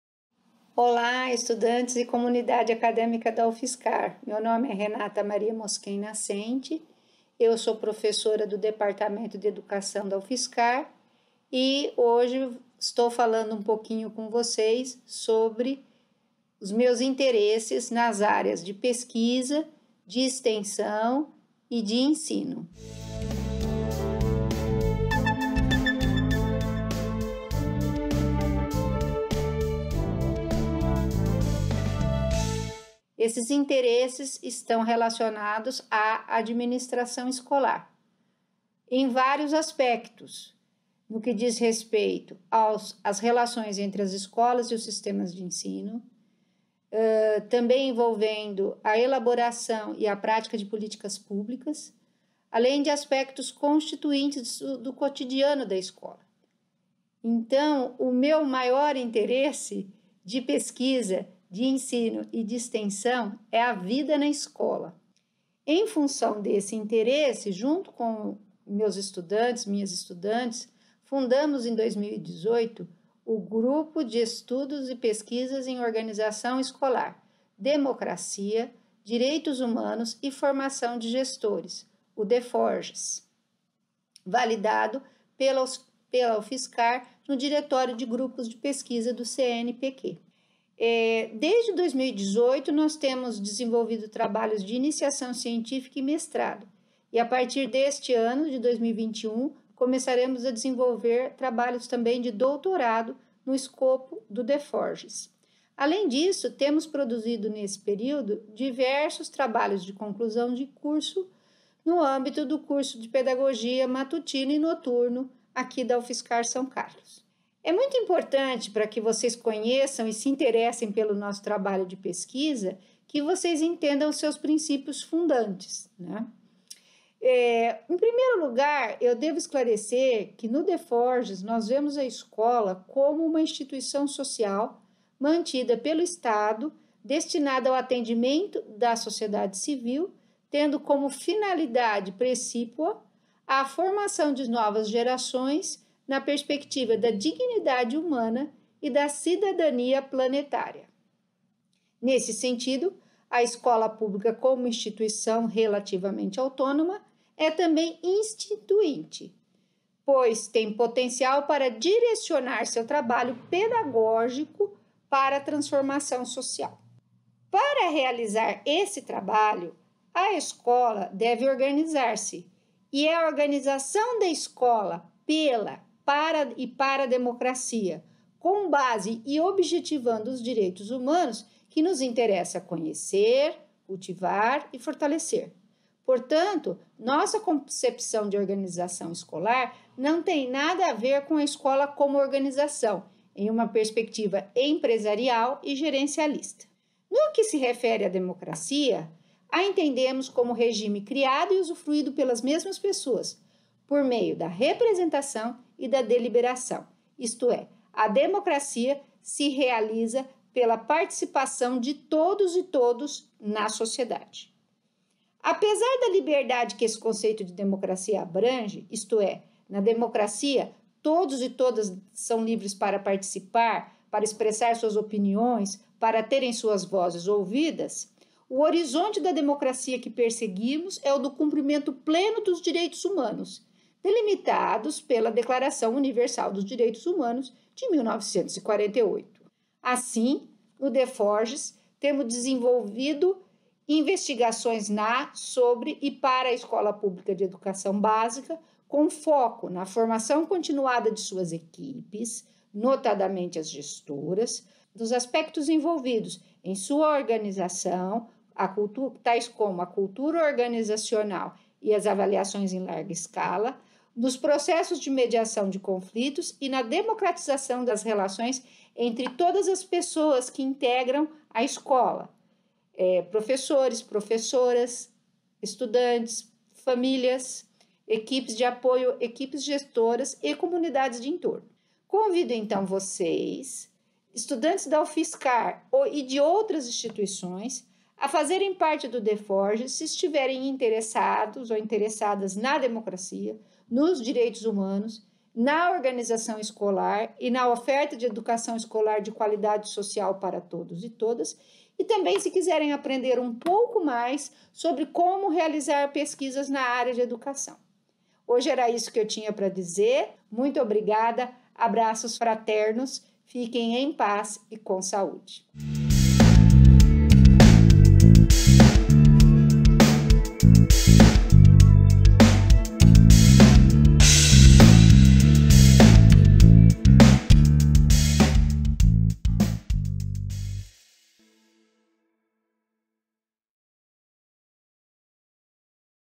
Organização escolar, democracia e direitos humanos são tema de pesquisa na UFSCar
Renata Maria Moschen Nascente, docente do Departamento de Educação da Universidade Federal de São Carlos (DEd - UFSCar), fala de suas pesquisas sobre administração escolar envolvendo políticas públicas e o próprio cotidiano das escolas. Seu grupo de pesquisa DEFORGES (Grupo de Estudos e Pesquisas em Organização Escolar: Democracia, Direitos Humanos e Formação de Gestores) é voltado à investigação da organização educacional e escolar, da formação de professores e da melhoria do espaço escolar público enquanto espaço de deliberação.